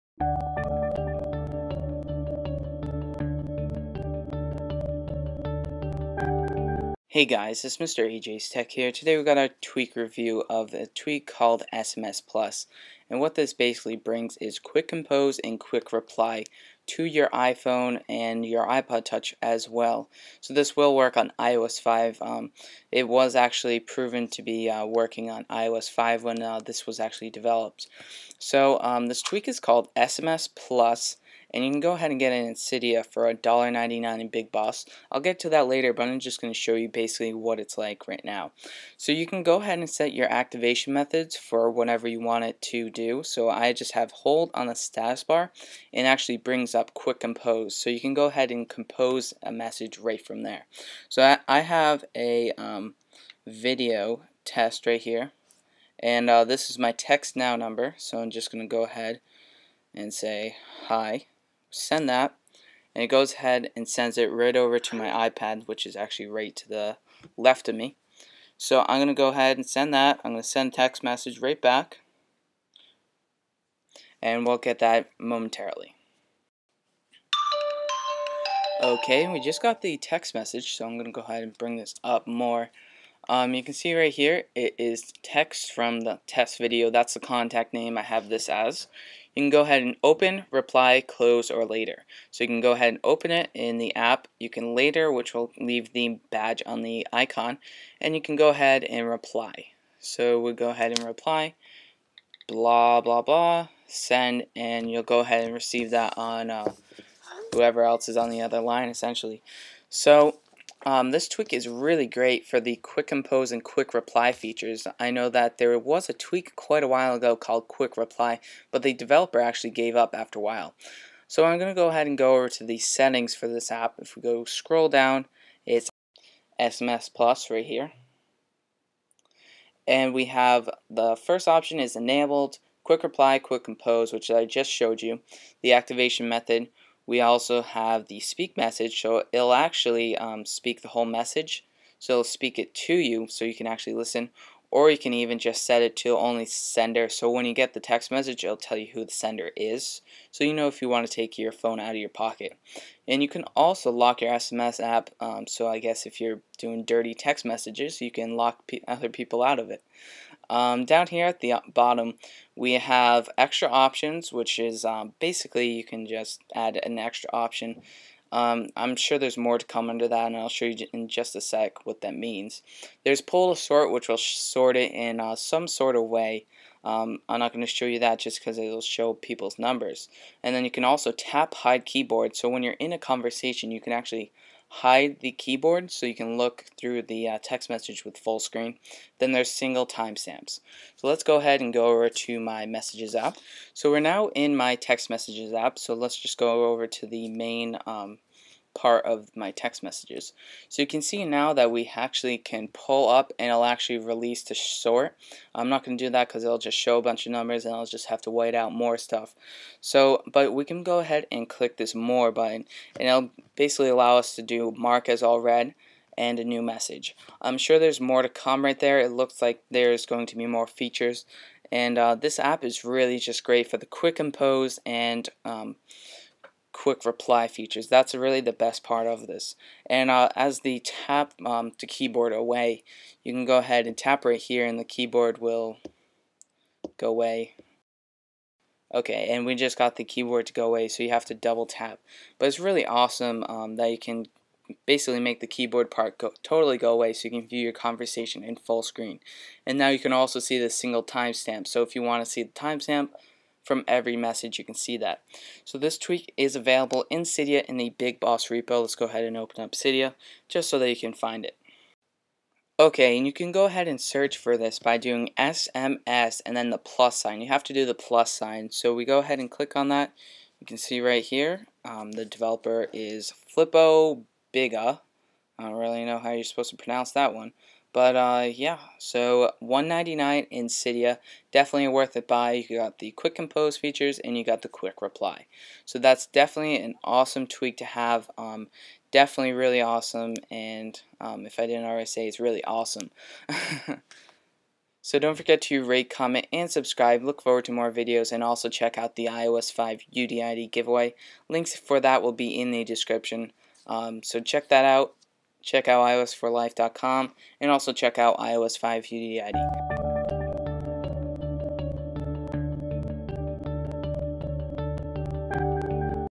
SMS+ QuickReply Cydia Tweak-iOS4 and iOS 5
SMS+ Brings Quick Compose and Quick Reply features to the SMS app on any jailbroken device from 4.3.3 to 5.0. It also adds features like: single time stamp, sorting, tap keyboard away, and more!

Better than BiteSMS and SwirlyMMS!!!

This tweak can be found in Cydia on the Bigboss repo. $1.99.

If you Liked the video be sure to thumbs up and comment!